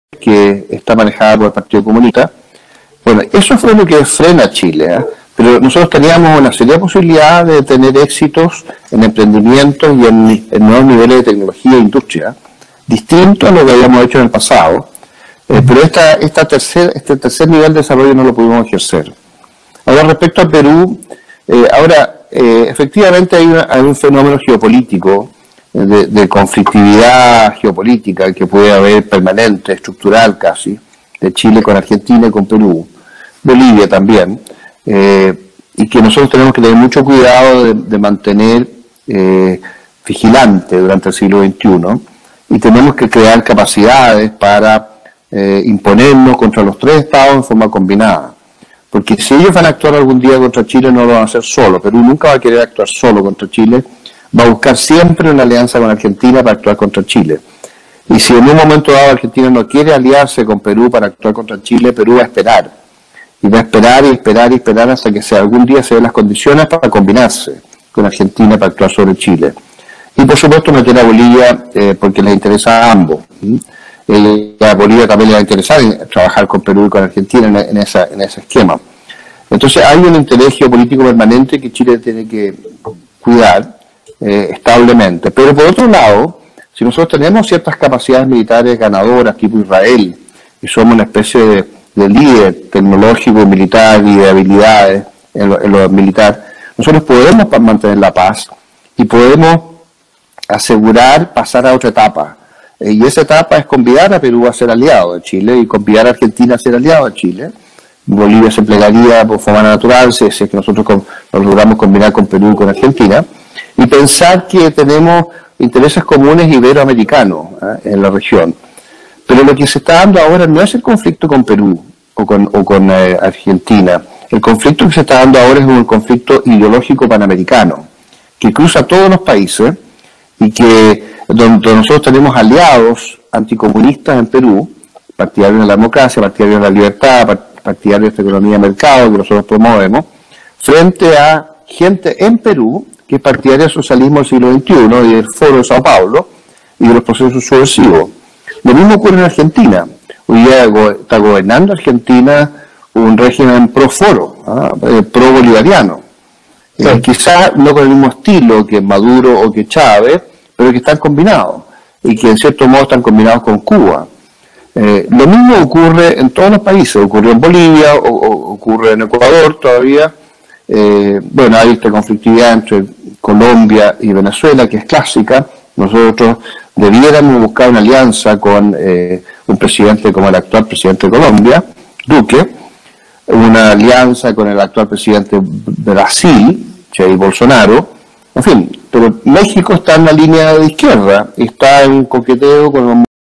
Perú Nunca va a Querer Actuar Solo Contra Chile
Va a esperar y esperar hasta que algun dia se den las condiciones.
Palabras de Daniel Prieto Vial
Defensape